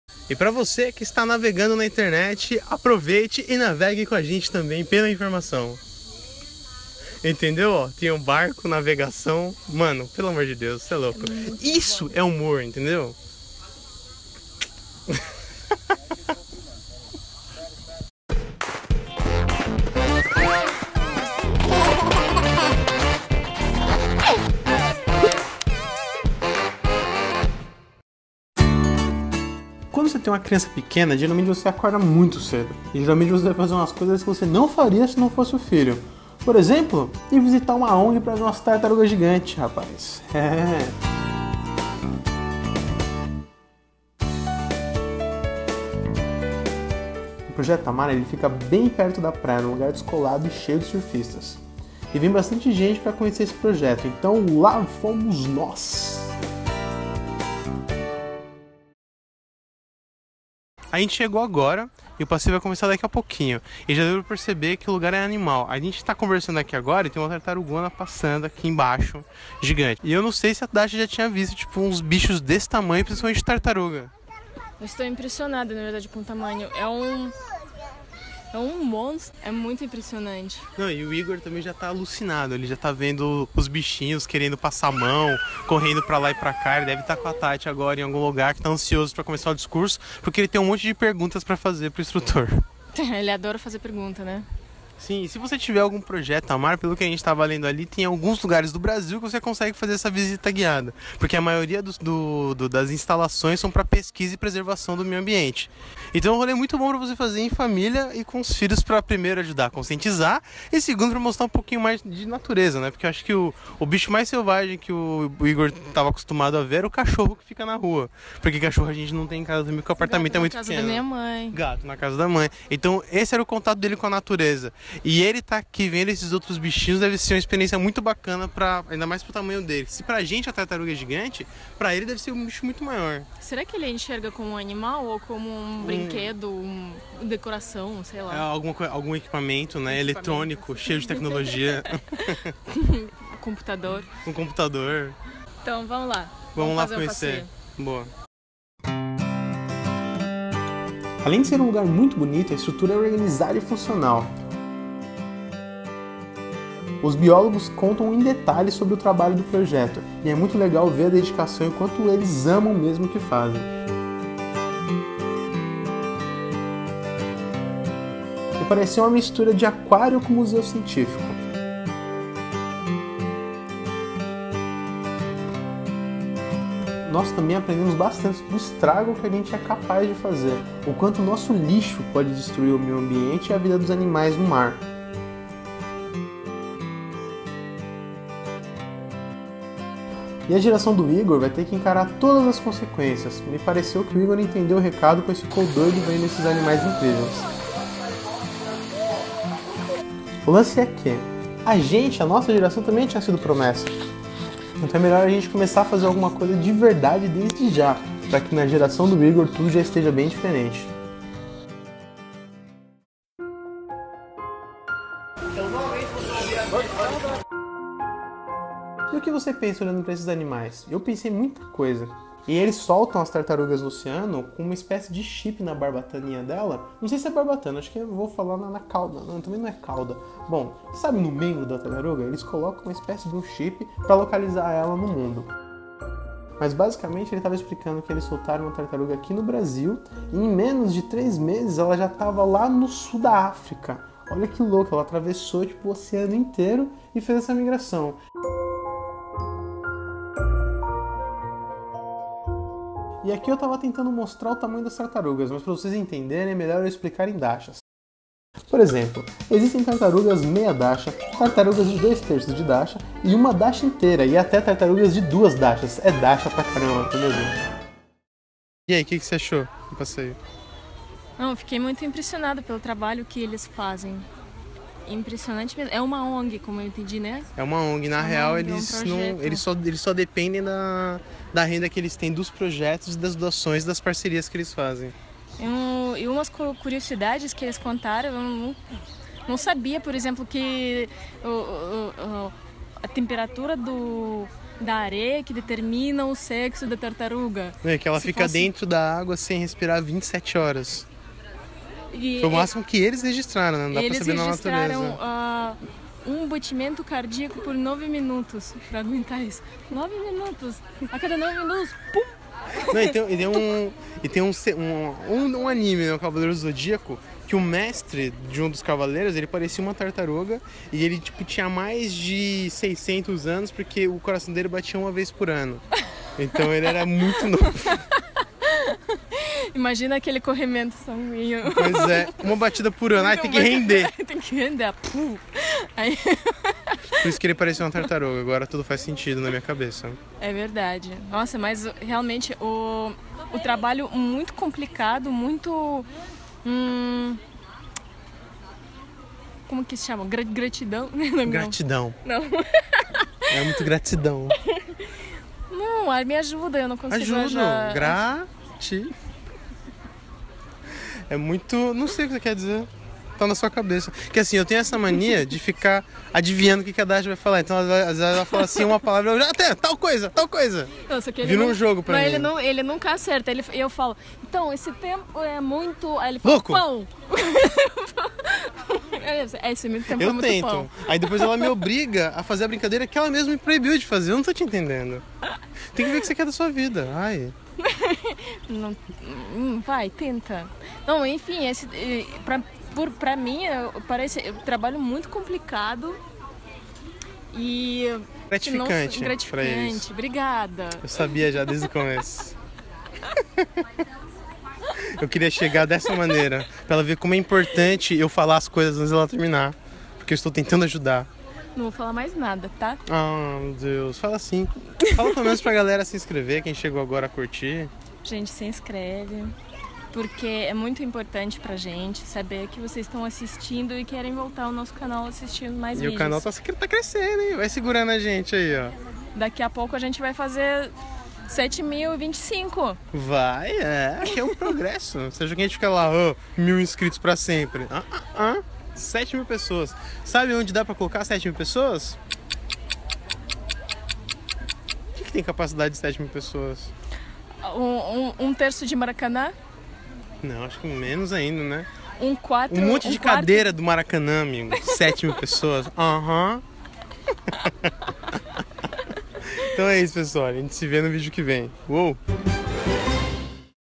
Tartarugas gigantes e briga de casal! - Ep. 22
Vem conhecer o Projeto Tamar junto com a gente!!!